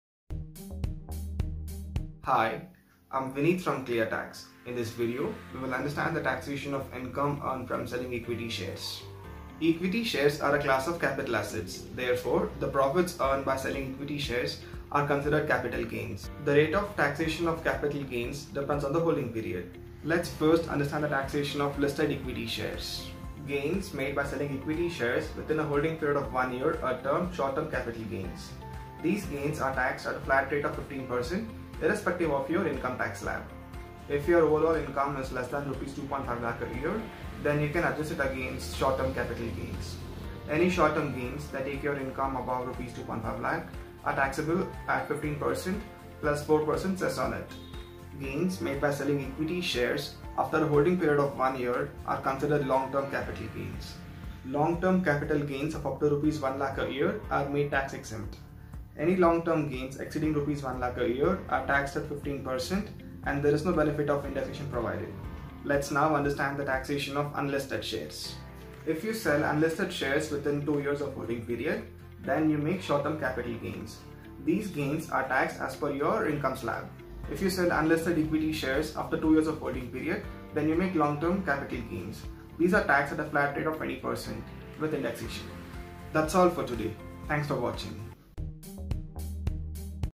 Income tax on Sale of Shares | Taxation on Long & Short term Capital Gains | Equity Shares
Equity shares are a class of capital assets. Hence profits made by selling shares are considered capital gains. The rate at which capital gains are taxed depends on the holding period.

Gains made through selling equity shares within one year are termed 'short-term capital gains', and are taxed at the rate of 15%. Gains made after a holding period of one year are considered 'long-term capital gains', and are taxed at 10% if they are above Rs 1 lakh a year.